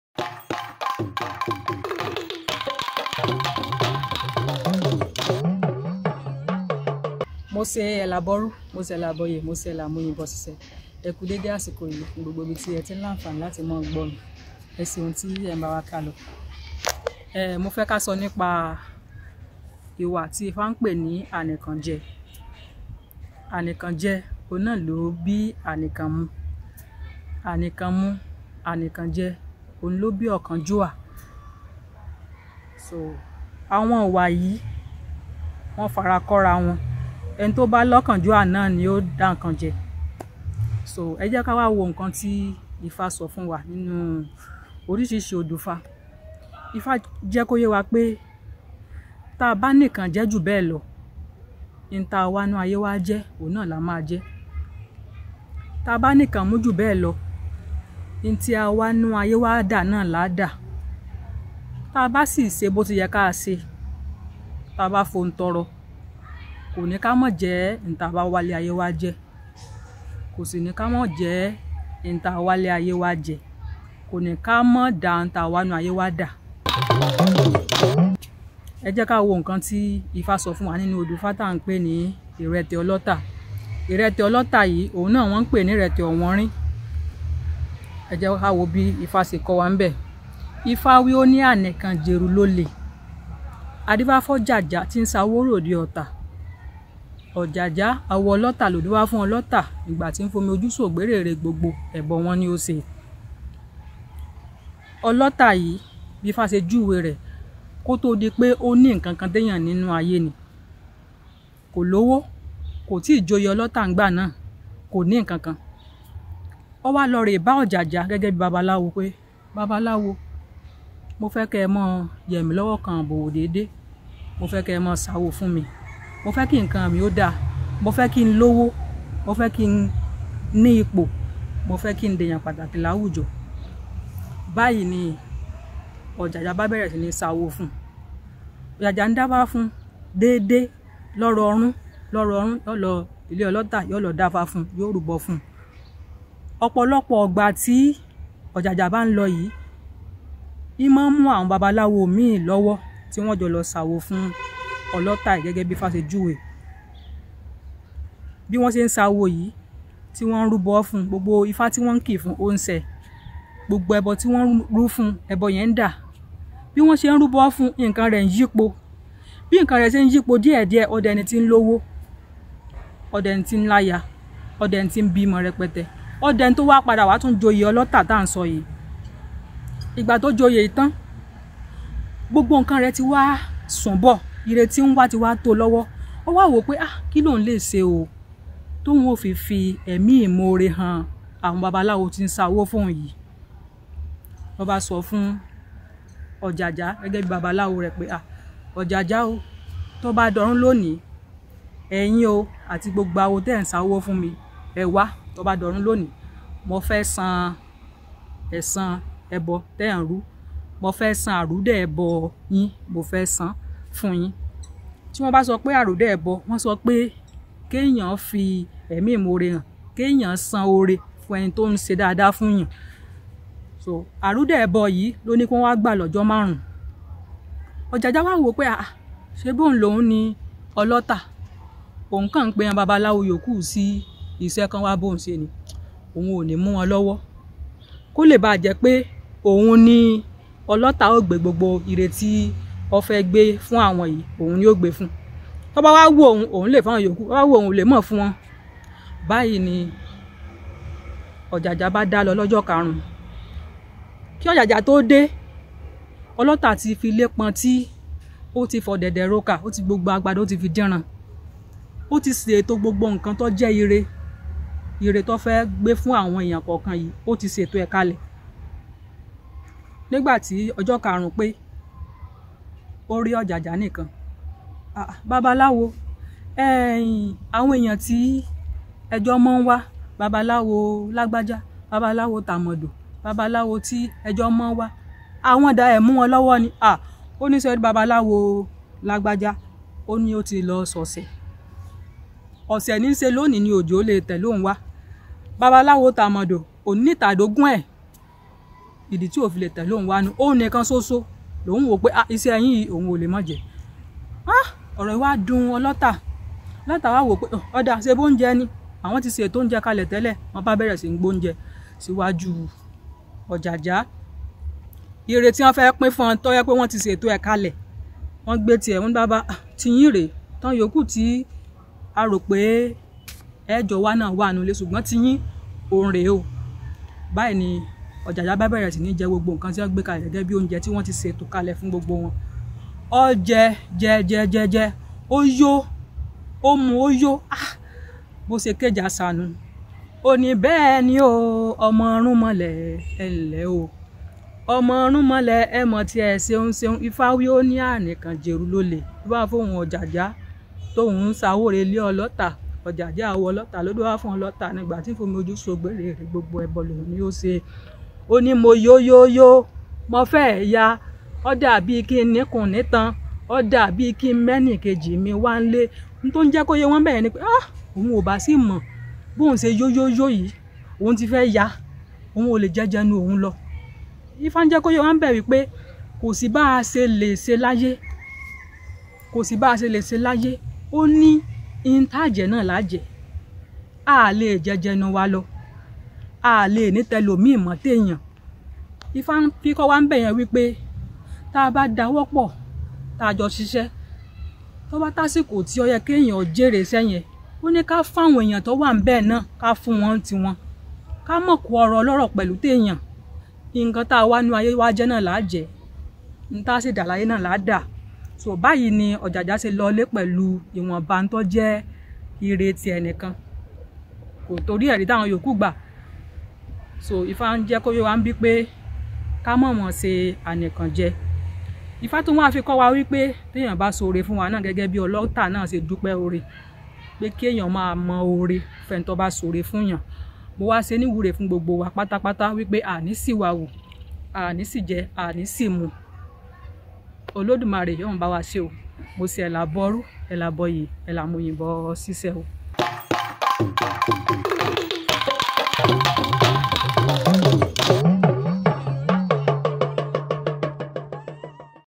The Ideological stance of Ifa on greediness by Iyanifa Ifaninyo
This video explains in 7 minutes what Ifa says about greediness.

Iyanifa Ifaninyo is a well-respected Ifa priestess; she's Ohun Olodumare TV's CEO.